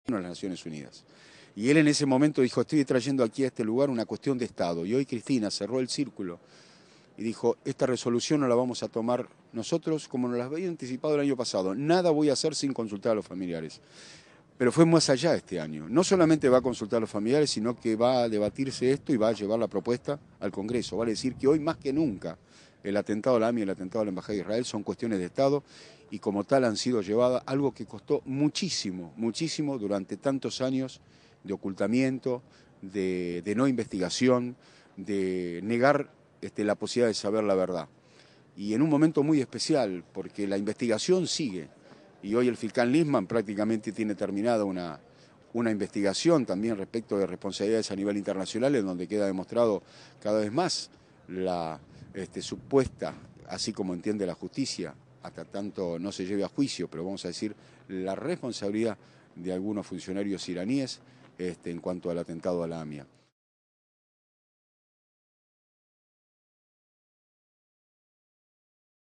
Burstein en ONU: "Cristina nos aseguró que no tomará desiciones sin consultar a los familiares"
Al asistir a la asamblea de la Organización de las Naciones Unidas (ONU), donde presenció el discurso de la presidenta Cristina Fernández de Kirchner, Sergio Burstein, titular de familiares y víctimas del atentado a la AMIA.  Leer más
Seguinos: